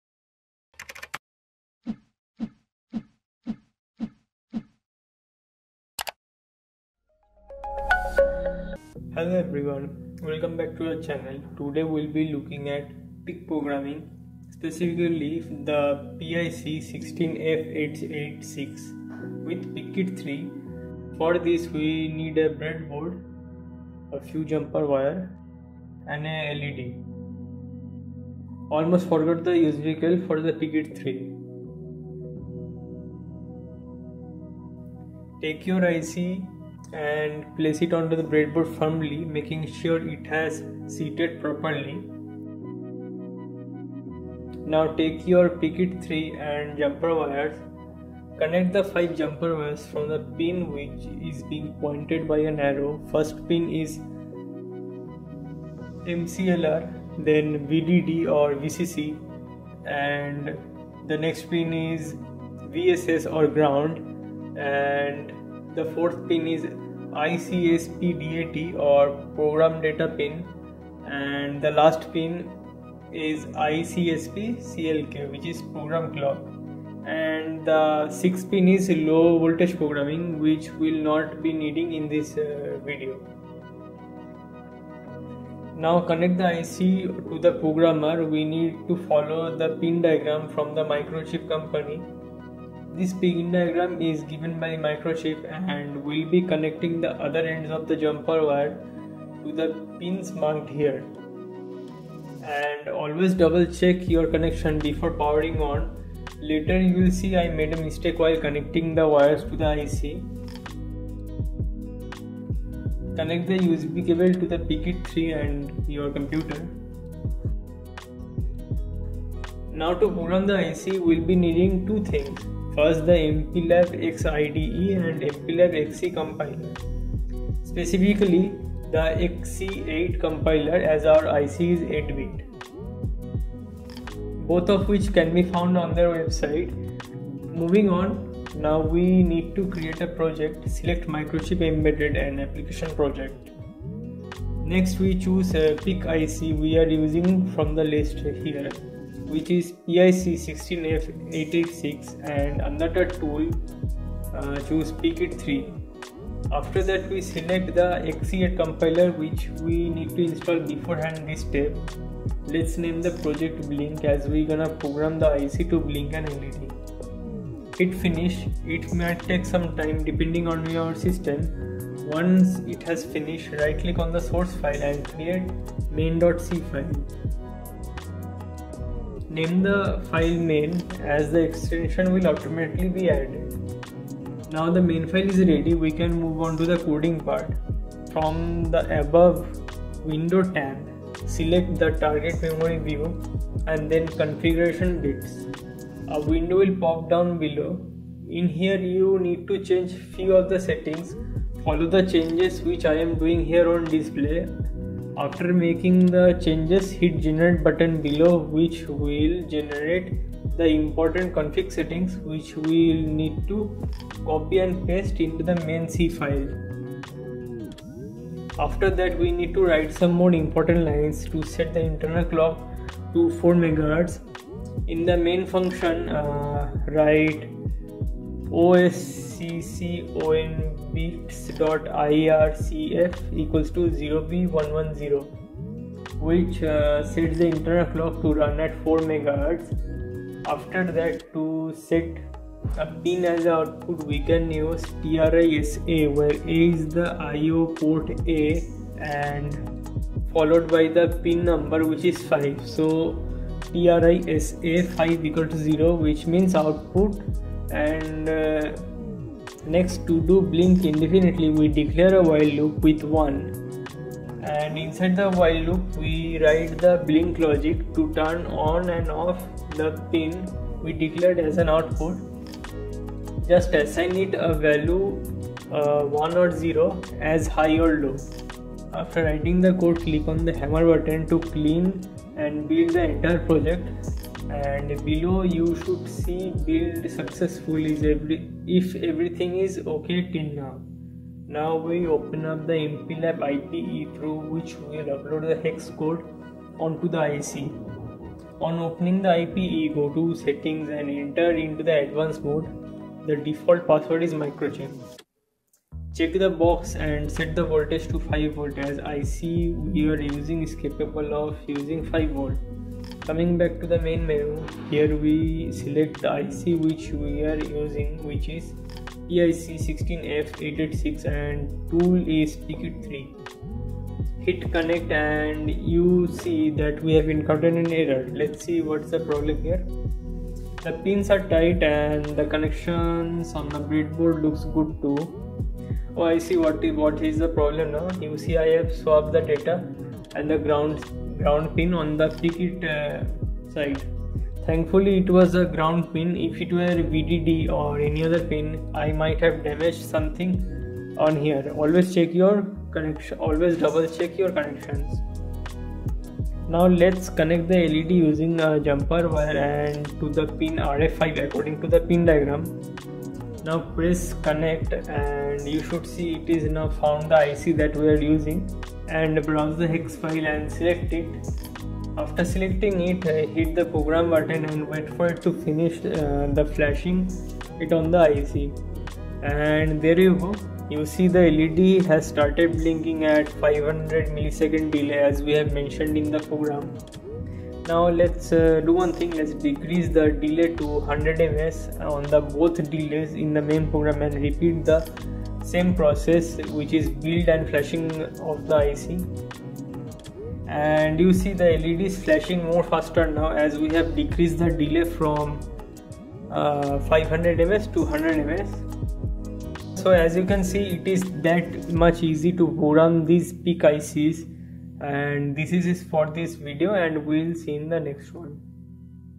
What is PIC Microcontroller? | Application and Blink Example
Hello IC enthusiasts,
In this video, we are going to understand PIC chips, how they work and where they can be used and show you a blink example.

A PIC stands for Peripheral interface controller may be a microcontroller which is intended by Microchip Technology. Initially, it was brought up as a Peripheral interface controller but over time, it's currently mentioned as Programmable Intelligent Computer.

The large reason PIC of holding a market is it's compatible with both hobbyists and industrial developers.

Applications:-
 1. Smartphones
 2. Medical devices
 3. Telecommunication devices and lot more